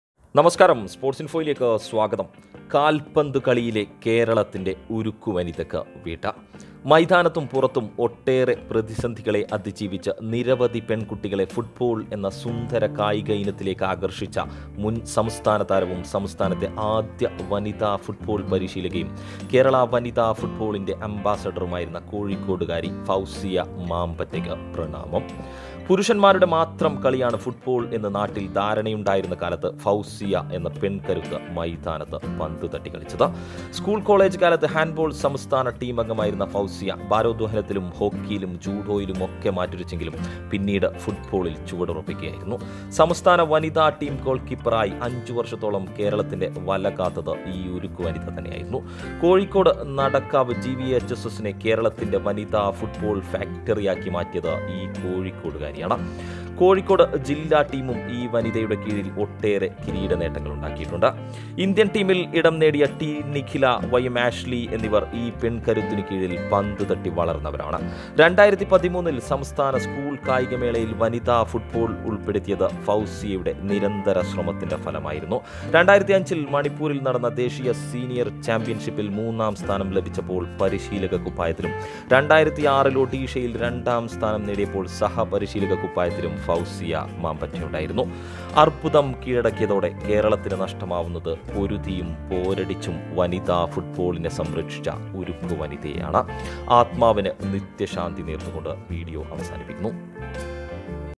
കേരള ഫുട്ബാളിന്റെ ഉരുക്കു വനിത 🌹
Former footballer and Kerala's first female football coach Fousiya Mampatta passed away. The 52-year-old was working as coach at Nadakkavu School. She was fighting cancer.
A native of Vellimadukunnu in Kozhikode, Fousiya took to the field at a time when girls were not even allowed to go to study, let alone play. She was the Goalkeeper for Kerala womens football team during the national games and All India Junior Women's Championship.